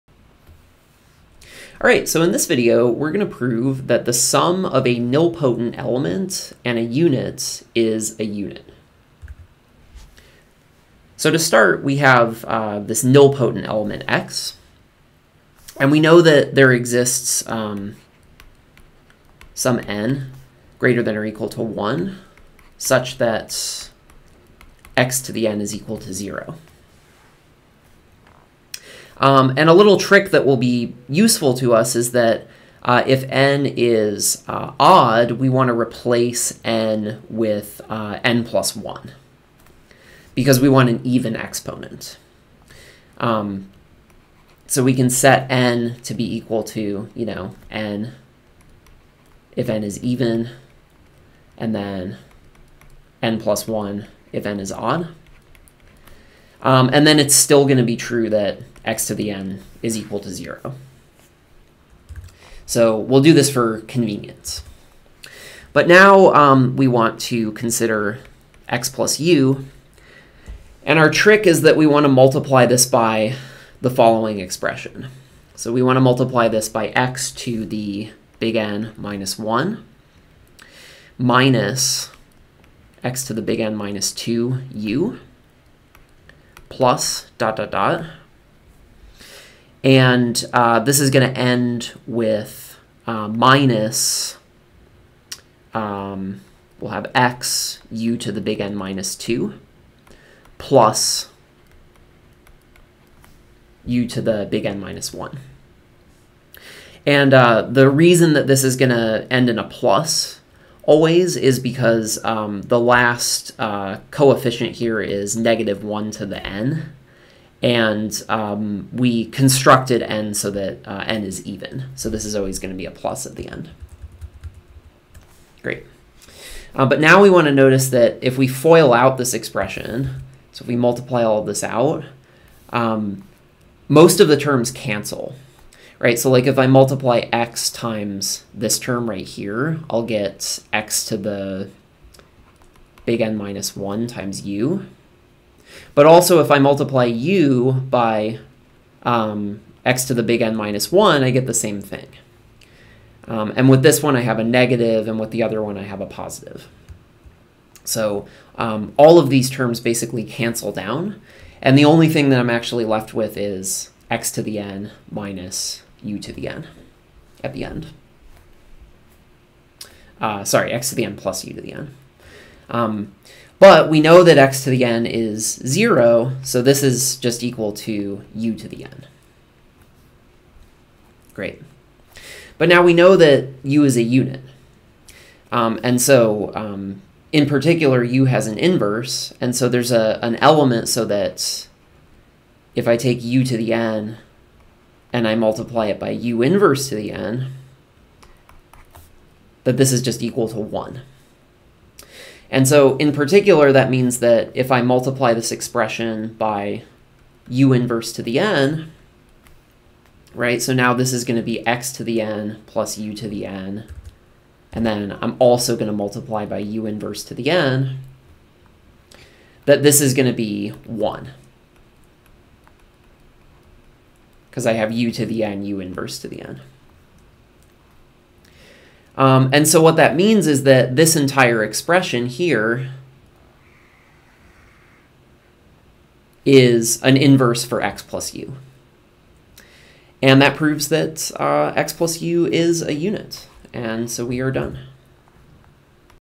The sum of a nilpotent and a unit is a unit
The sum of a nilpotent and a unit is a unit (we are assuming R is a commutative ring with identity).